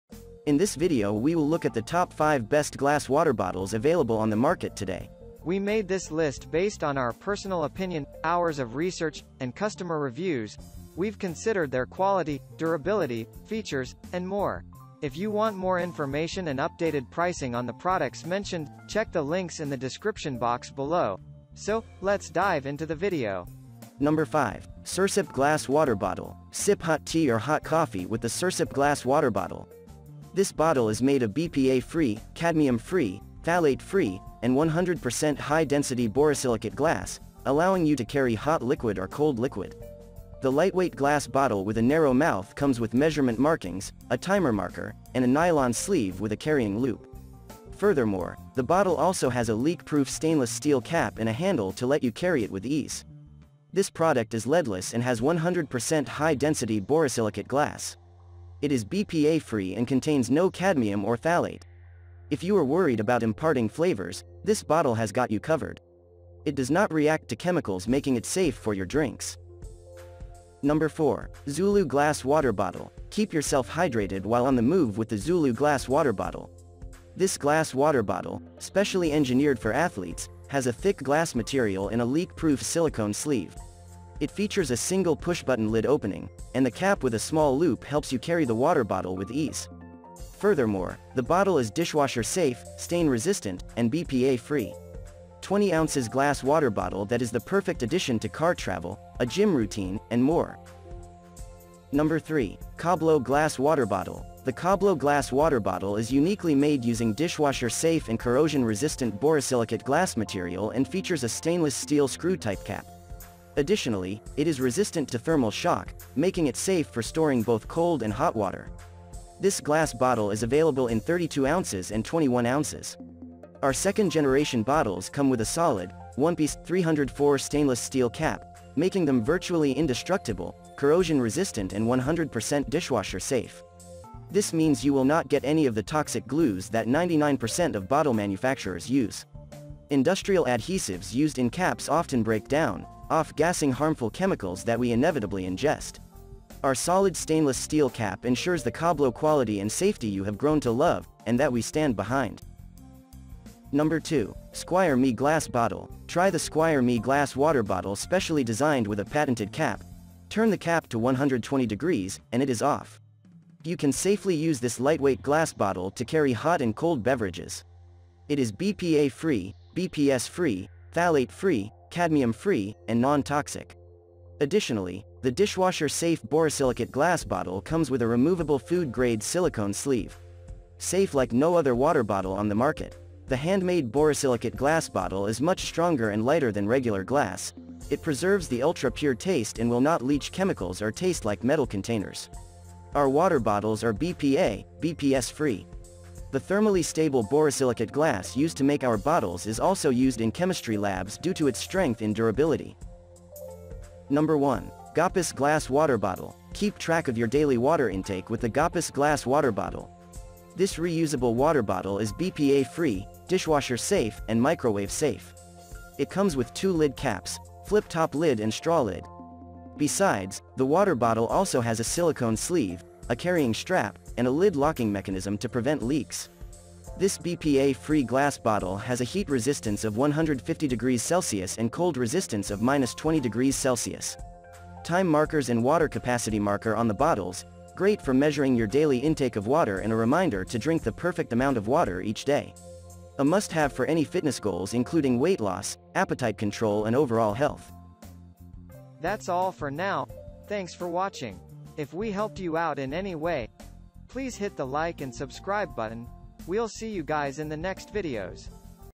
Top 5 Best Glass Water Bottles In 2022
☛ All The Links to Best Glass Water Bottles listed in this Video:-

►5. Sursip Glass Water Bottle.

► 4. Zulu Glass Water Bottle.

► 3. Kablo Glass Water Bottle.

► 2. SquireMe Glass Bottle.

►1. Goppus Glass Water Bottle.